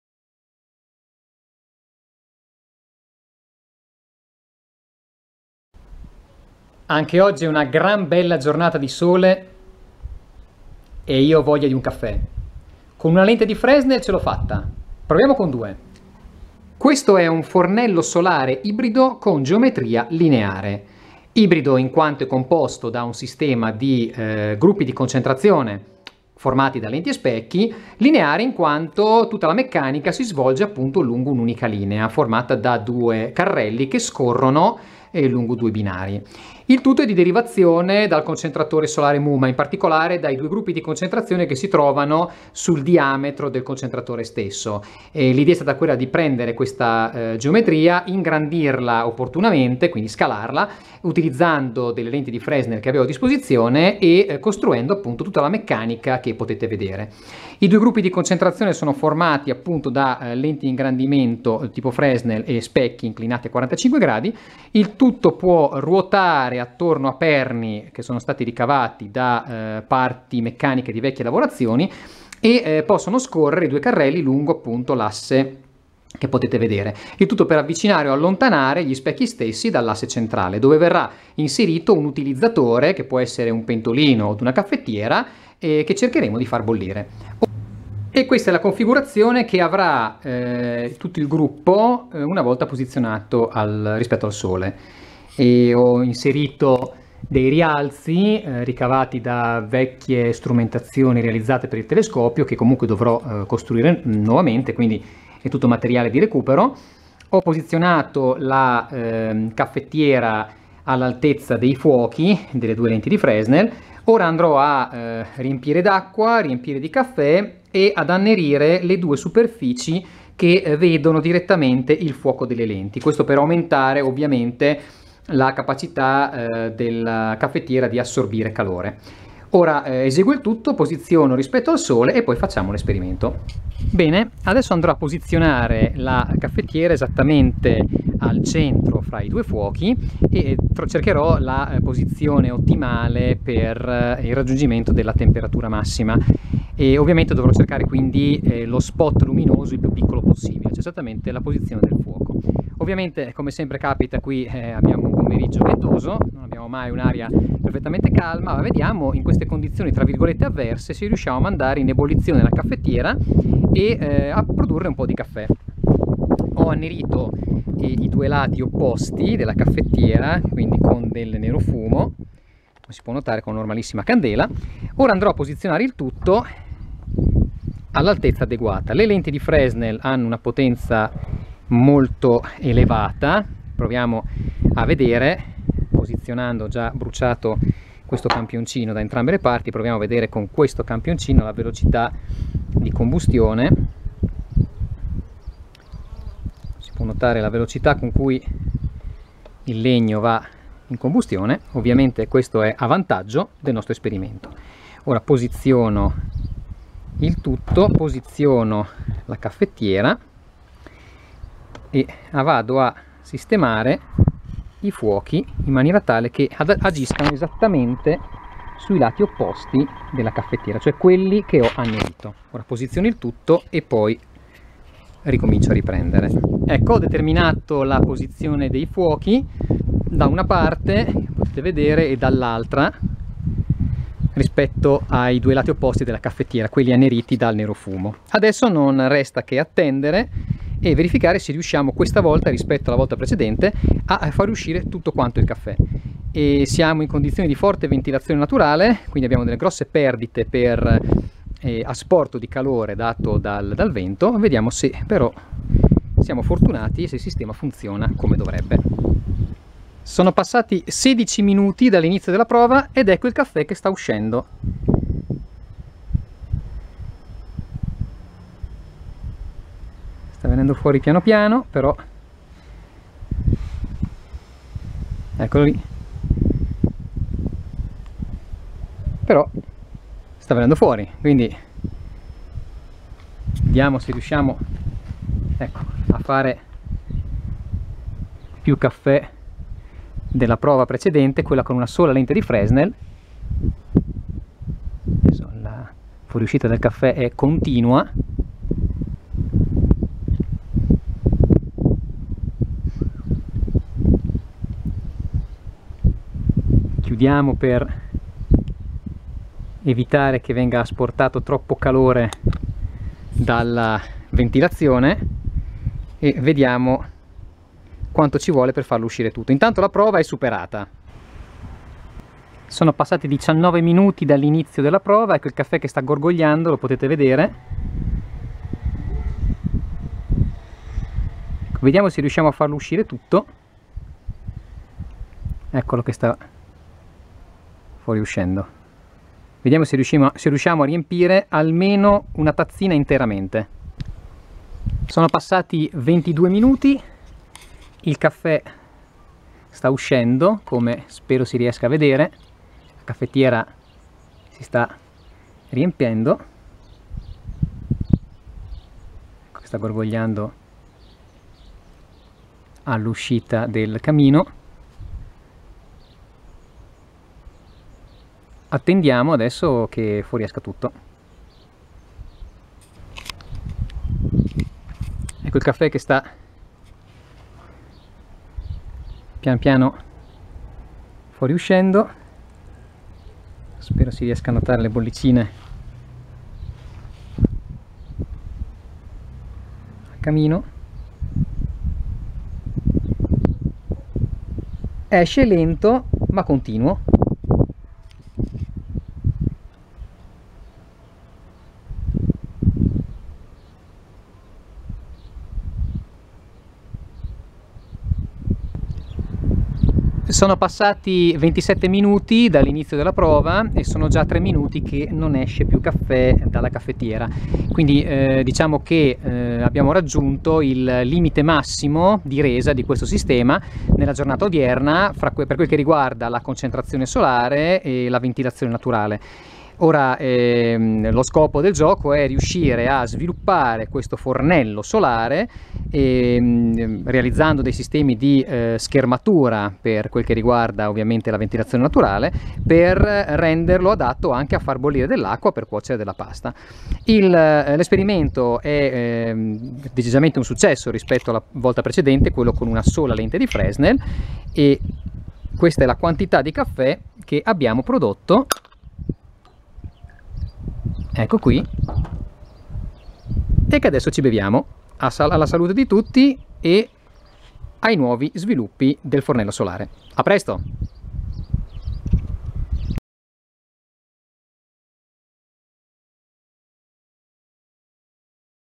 Fornello solare MUMA - MUMA Solar Cooker
Prototipo di fornello solare

N° lenti di Fresnel utilizzate = 2
Sup. Captante = 0.1176 mq
Potenza in ingresso = 117.6 W